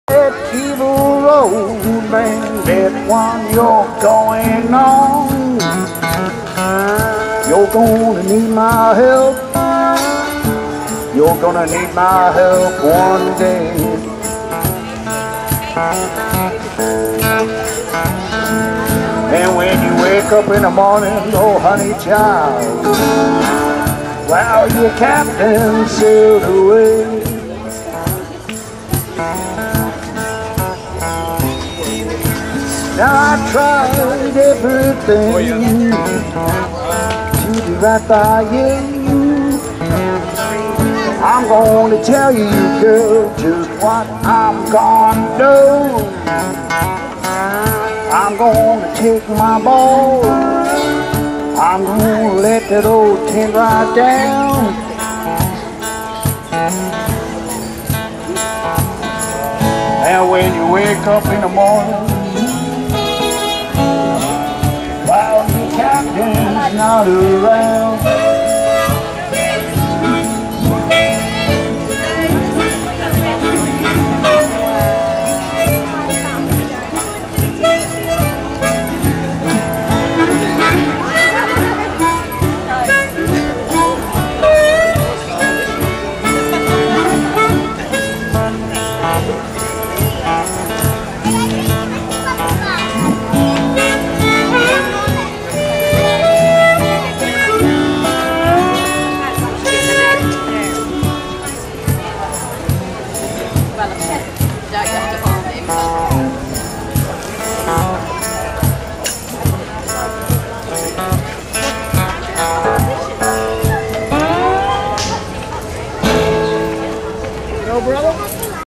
Captain Tricko at Eumundi markets Queensland Australia
Captain entertains with his one man band.